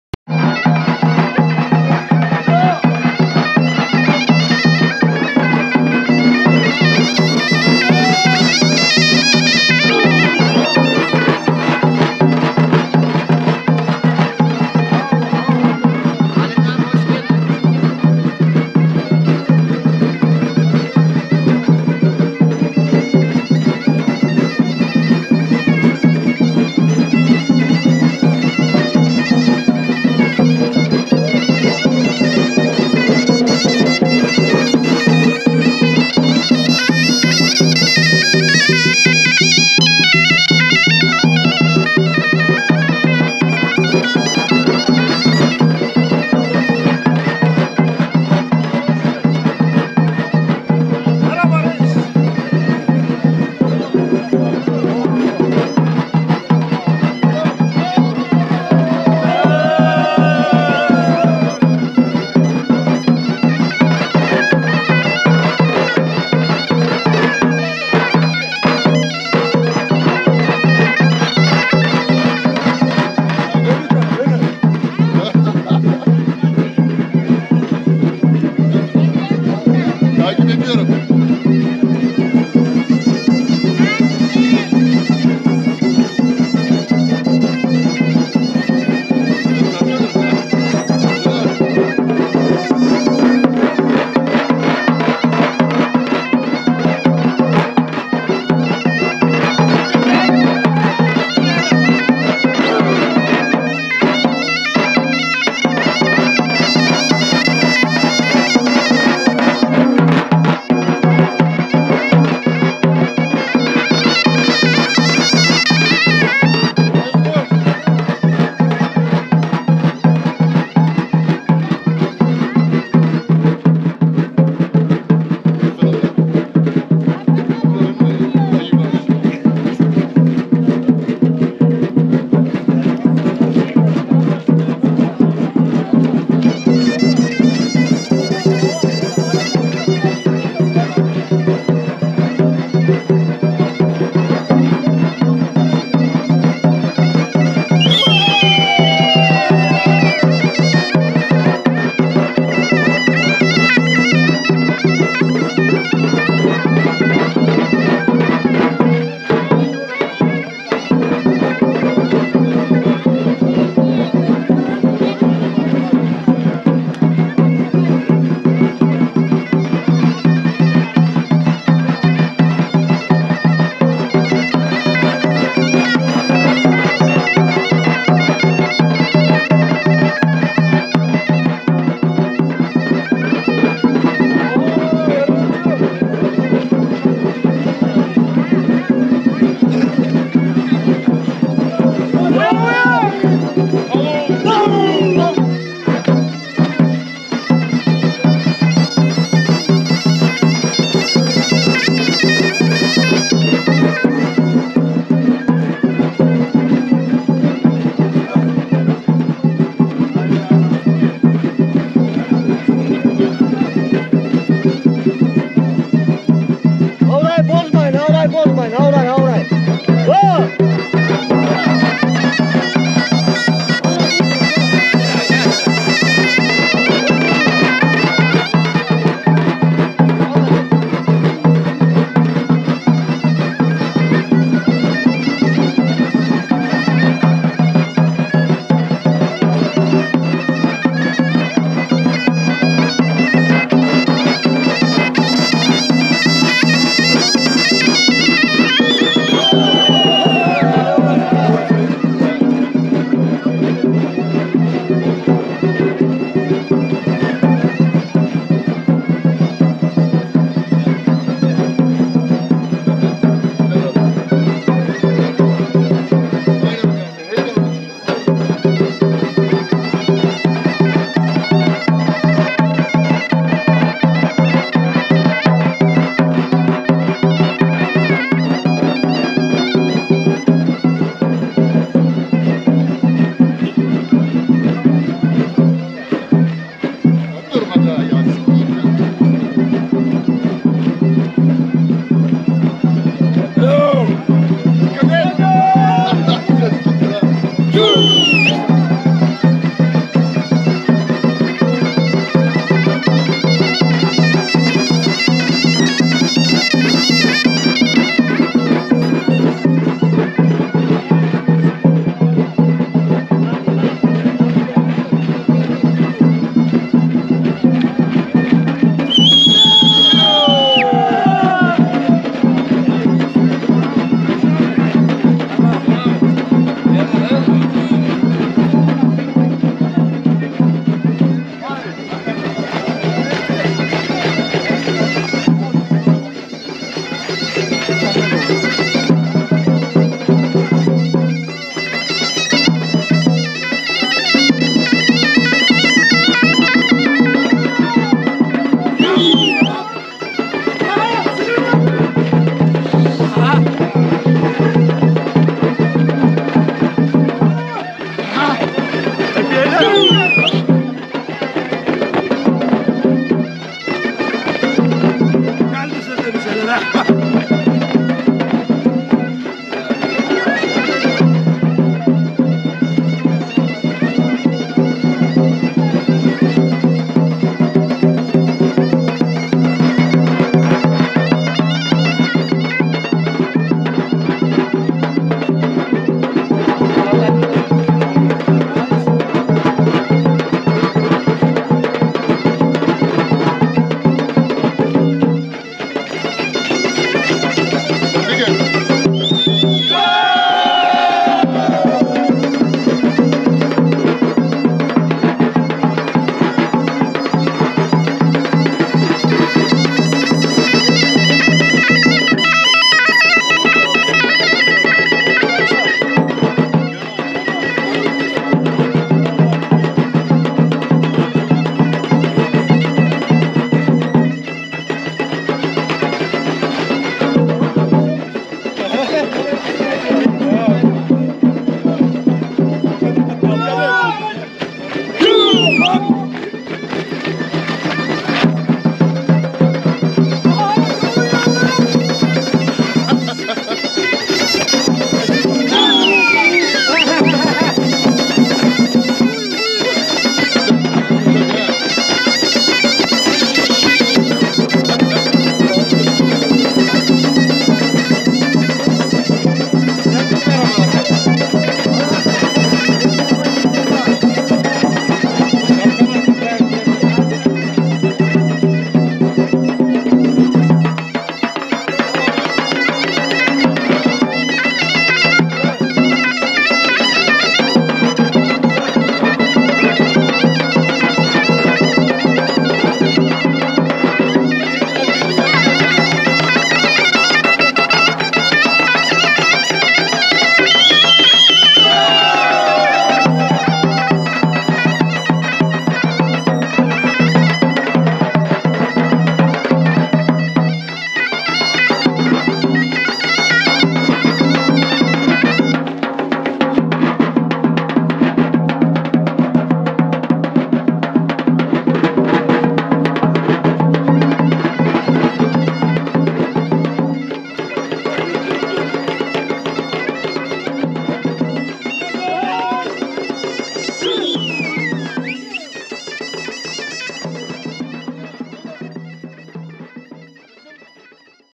Hıdırnebi Şenlikleri - Harika Bir Horon...
Trabzon Akçaabat Acısu Mahallesinden selamlar. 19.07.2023 Hıdırnebi çimeni ve harika bir horon...